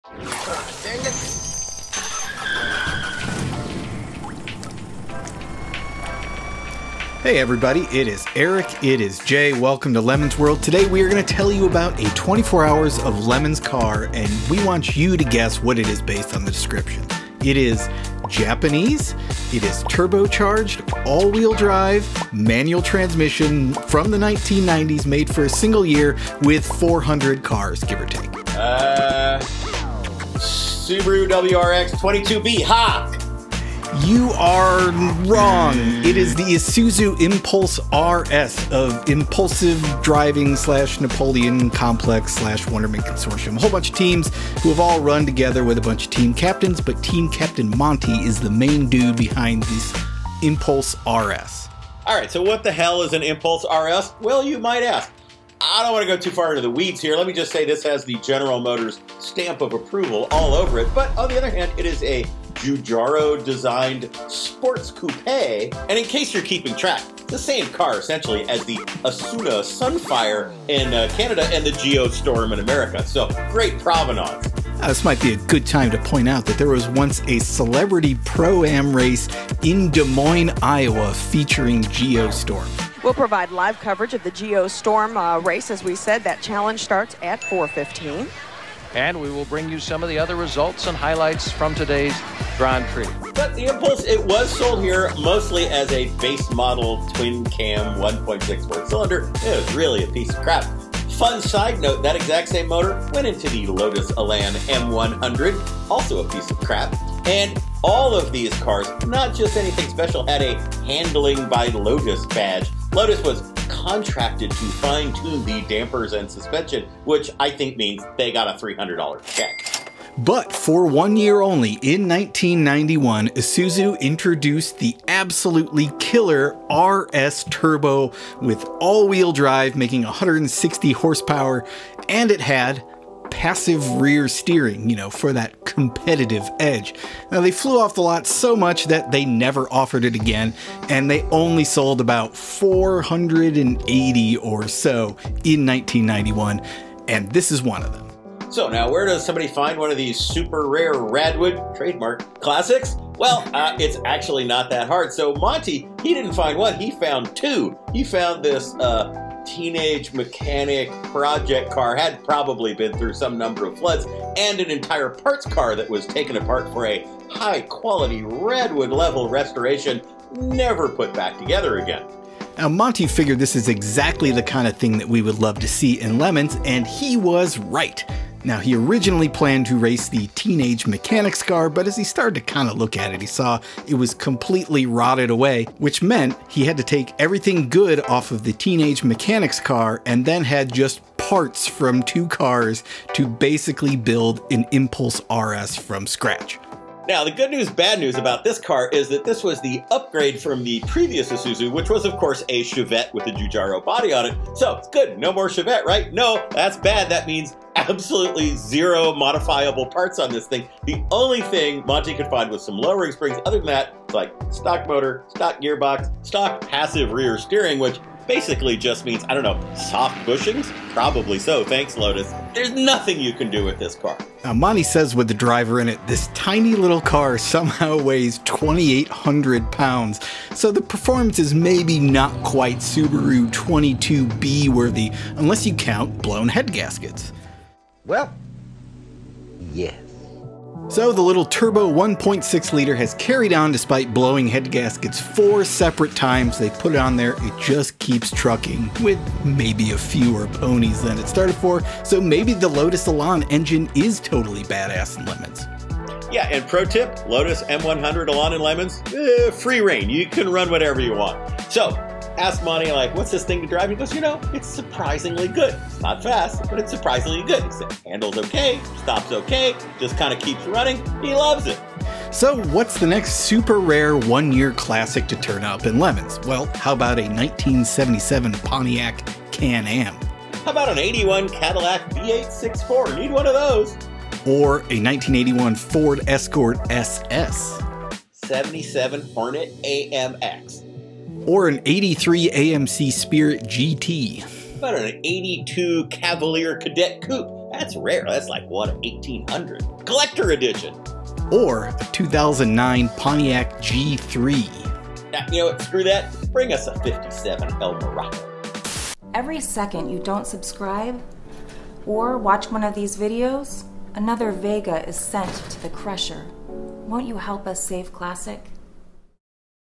Turbocharged Japanese AWD From '91 - lemonsworld 178
Jay and Eric discuss a Japanese turbocharged car that was made for one year only in 1991. Can you guess what JDM car it is?